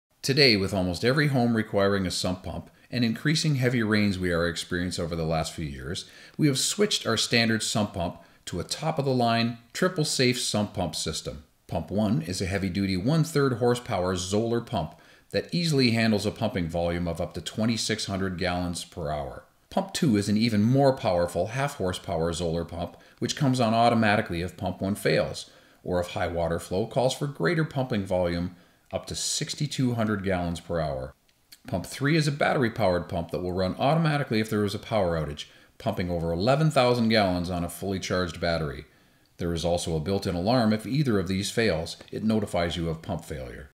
Gatta Homes Sump Pump
Homes of Elegance offers an exclusive triple safe sump pump system. It has a 3 tier system that assures the safety of your sump pump.